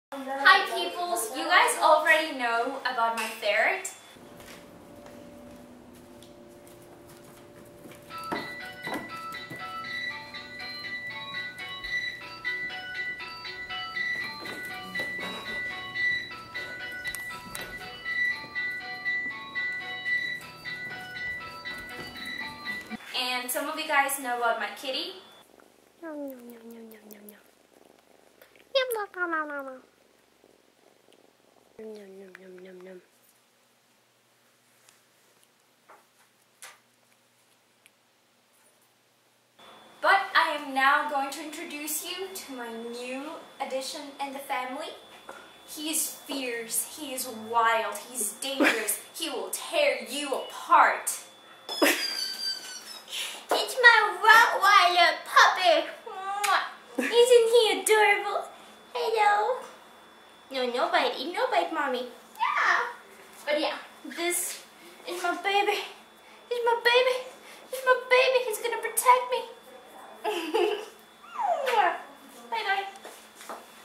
My pets ♥
Meet Kyler my ferret, Oliver my kitty, and Skylar my rottweiler